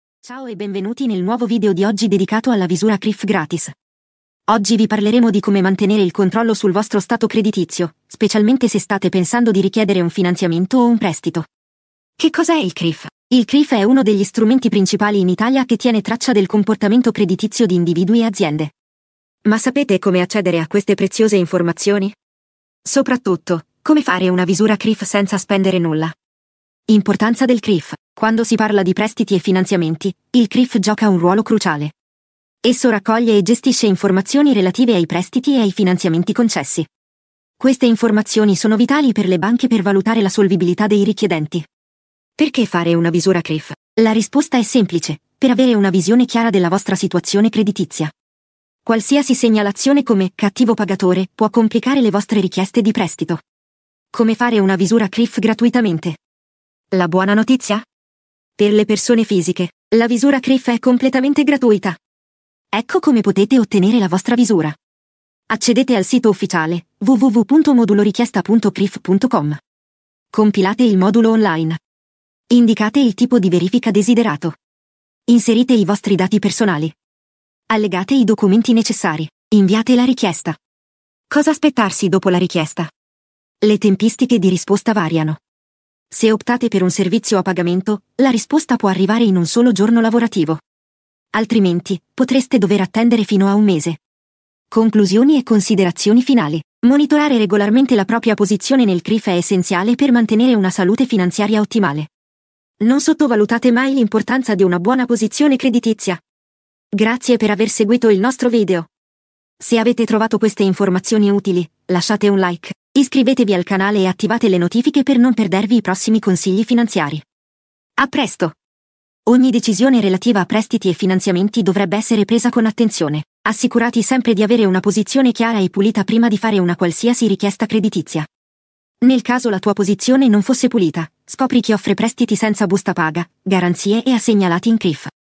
Come Effettuare una Visura CRIF Gratuitamente: Guida Definitiva
.: 🎥 Introduzione:

Benvenuti, cari spettatori! Oggi vi parleremo di come mantenere il controllo sul vostro stato creditizio, specialmente se state pensando di richiedere un finanziamento o un prestito.

🎥 Che cosa è il CRIF?:

Il CRIF è uno degli strumenti principali in Italia che tiene traccia del comportamento creditizio di individui e aziende. Ma sapete come accedere a queste preziose informazioni? E, soprattutto, come fare una visura CRIF senza spendere nulla?

🎥 Importanza del CRIF:

Quando si parla di prestiti e finanziamenti, il CRIF gioca un ruolo cruciale. Esso raccoglie e gestisce informazioni relative ai prestiti e ai finanziamenti concessi. Queste informazioni sono vitali per le banche per valutare la solvibilità dei richiedenti.

🎥 Perché fare una visura CRIF?:

La risposta è semplice: per avere una visione chiara della vostra situazione creditizia. Qualsiasi segnalazione come "cattivo pagatore" può complicare le vostre richieste di prestito.

🎥 Come fare una visura CRIF gratuitamente:

La buona notizia? Per le persone fisiche, la visura CRIF è completamente gratuita! Ecco come potete ottenere la vostra visura:

Compilate il modulo online.
Indicate il tipo di verifica desiderato.
Inserite i vostri dati personali.
Allegate i documenti necessari.
Inviate la richiesta.
🎥 Cosa aspettarsi dopo la richiesta?:

Le tempistiche di risposta variano. Se optate per un servizio a pagamento, la risposta può arrivare in un solo giorno lavorativo. Altrimenti, potreste dover attendere fino a un mese.

🎥 Conclusioni e considerazioni finali:

Monitorare regolarmente la propria posizione nel CRIF è essenziale per mantenere una salute finanziaria ottimale. Non sottovalutate mai l'importanza di una buona posizione creditizia!

Grazie per aver seguito il nostro video! A presto! 👋📈🏦